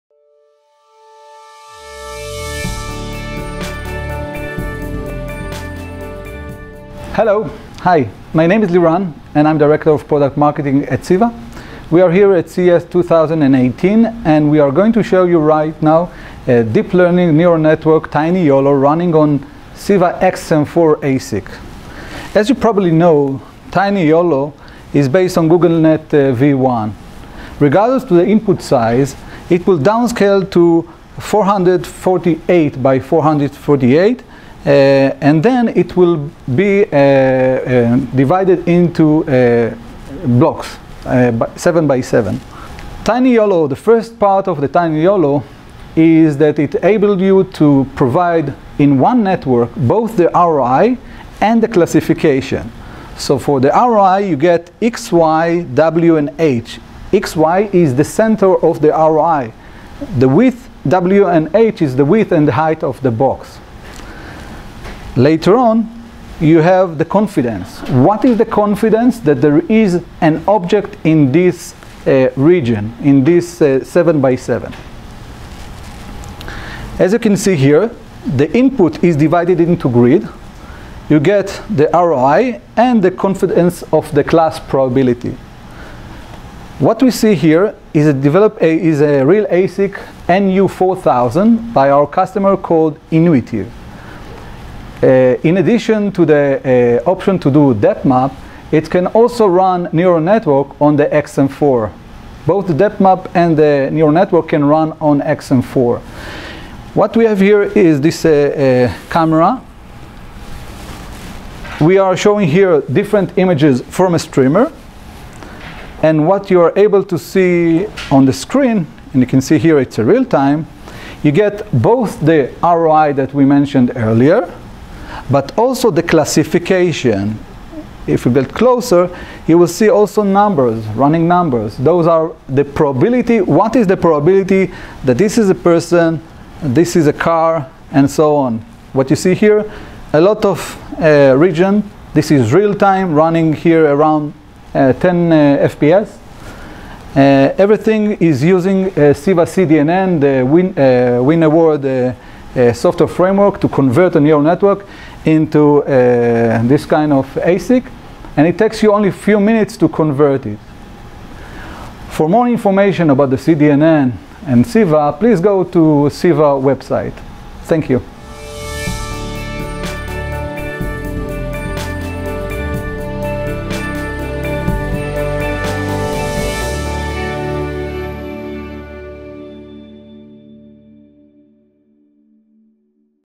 CEVA Demo of Real-time Tiny Yolo Neural Network on Inuitive NU4000 SoC Powered by CEVA-XM4
For more information about embedded vision, including hundreds of additional videos, please

Liran Bar, Director of Product Marketing at CEVA, demonstrates the company's latest embedded vision technologies and products at the January 2018 Consumer Electronics Show. Specifically, Bar demonstrates state-of-the-art deep learning technology in the form of a real-time Tiny Yolo neural network running on Inuitive's latest NU4000 SoC powered by the CEVA-XM4 vision processor.

The Tiny Yolo porting work to the CEVA-XM4 was performed by Inuitive's engineers, leveraging CEVA’s award winning CDNN software framework, and was completed in a very short time.